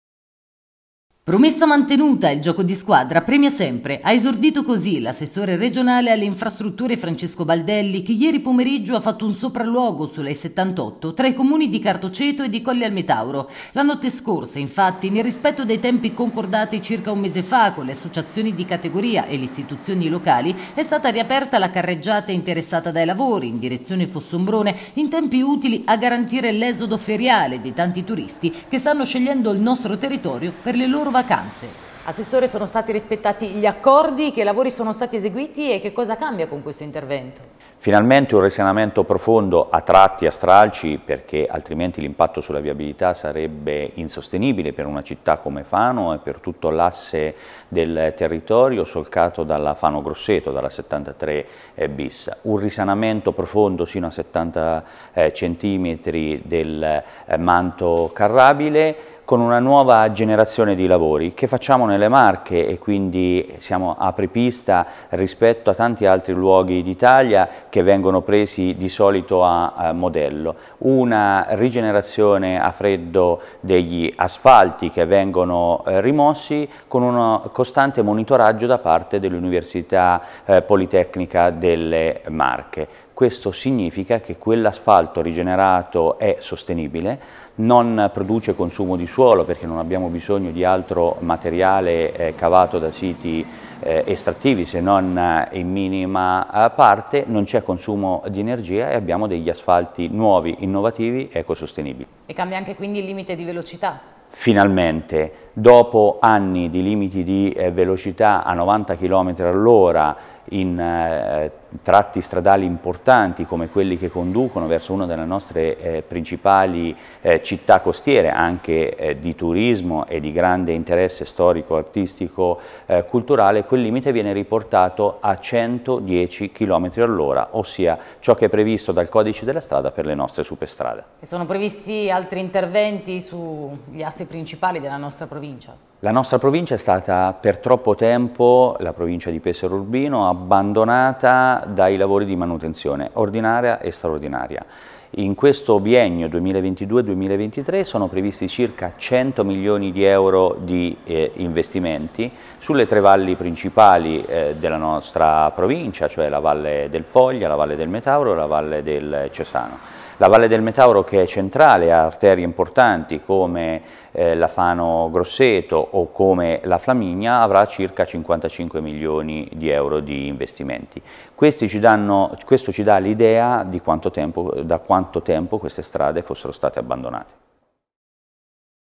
E78, Baldelli: "Riaperto tratto Lucrezia-Calcinelli"
Nuovo asfalto, tecniche all’avanguardia e limite di velocità che torna a 110km/h: l’assessore regionale Baldelli annuncia la riapertura del tratto di Superstrada tra Lucrezia e Calcinelli.